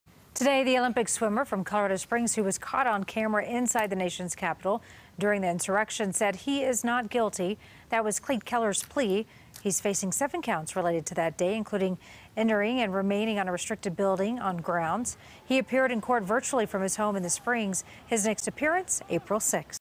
Klete Keller Pleads Not Guilty To Charges In Deadly U.S. Capitol Riot
Five-time Olympic swimming medalist Klete Keller has pleaded not guilty to charges that he participated in a deadly riot at the U.S. Capitol in January.